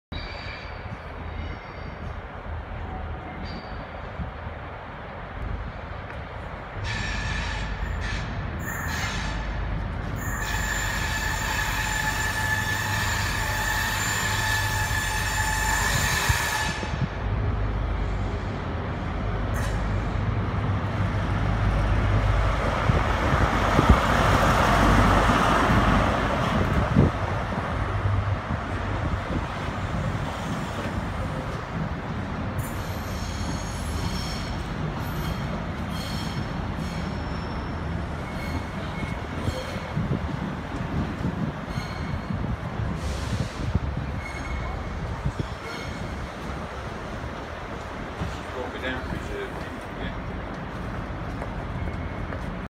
Virgin East Coast train arriving in York platform 1
Final calling point: London King’s Cross